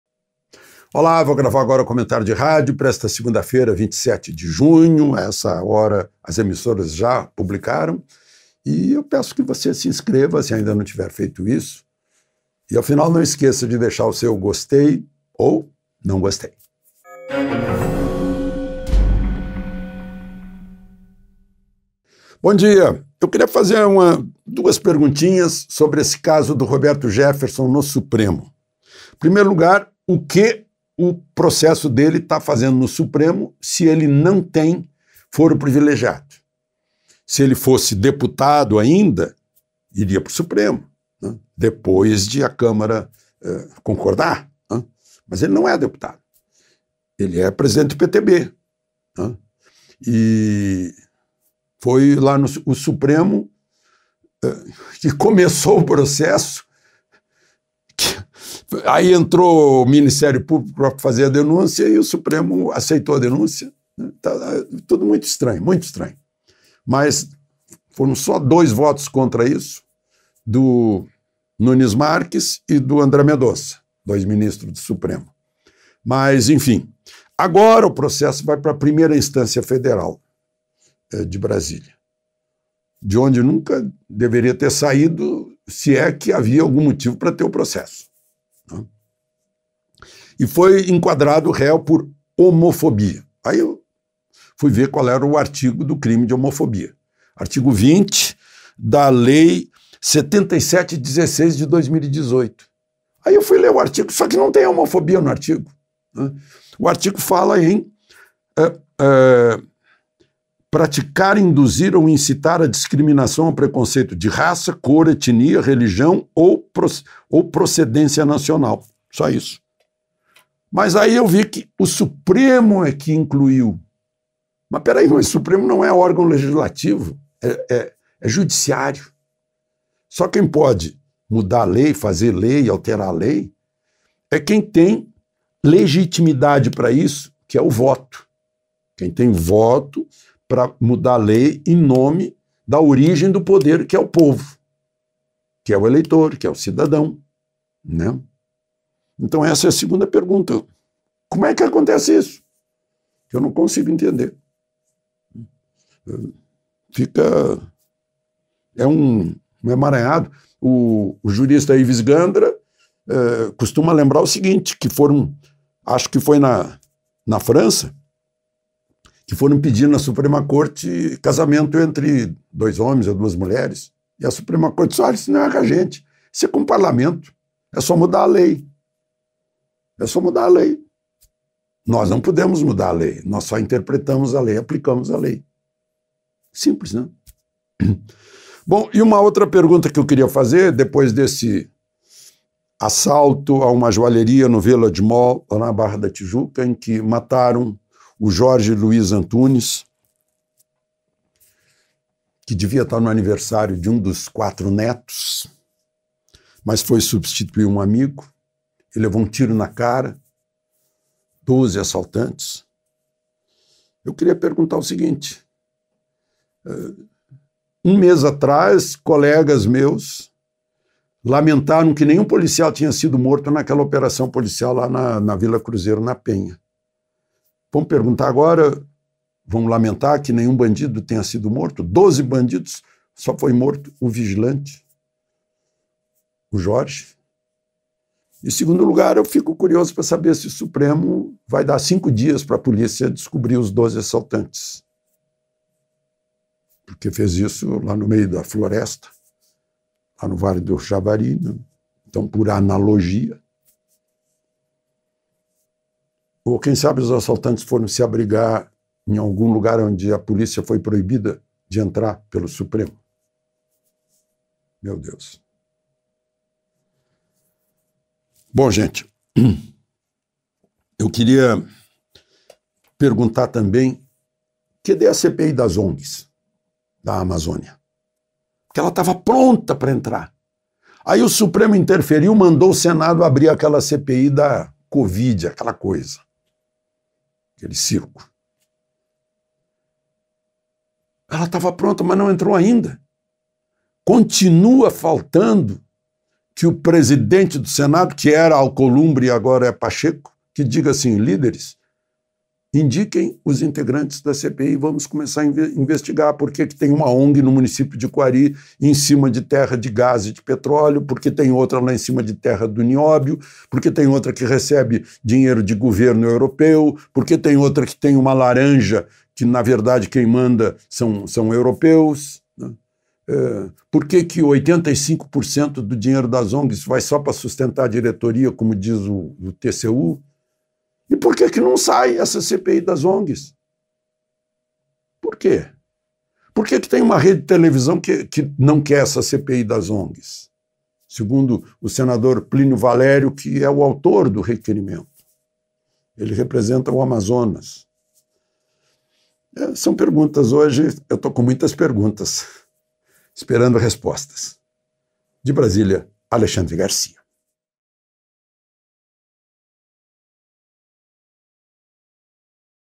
Em Jefferson, crime sem lei
Meu comentário desta segunda-feira em 300 rádios